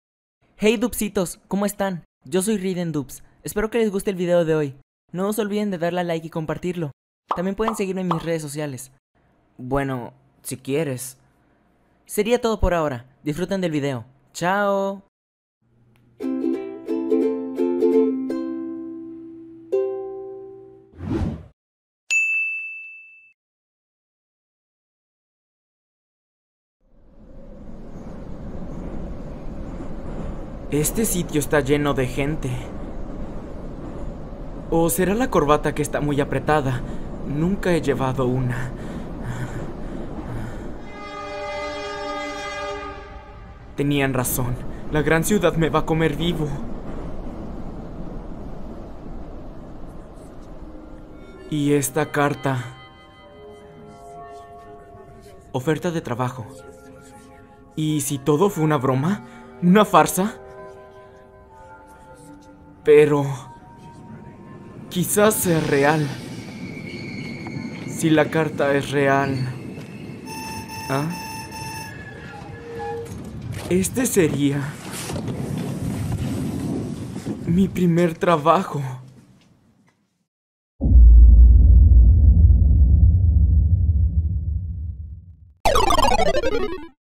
PRUEBA DE VOZ en Español||Brian Pasternack|Yuppie Spycho
LEE LA DESCRIPCIÓN. . .             (Gracias)
(Esto son pruebas de voz que hago de vez en cuando, para probar mi voz en distintos personajes, espero les guste.)

JUEGO: Yuppie Spycho

VOZ POR:
-ReidenDoesStuff

Este video tiene la finalidad de entretener, esta hecho de un fan para fans.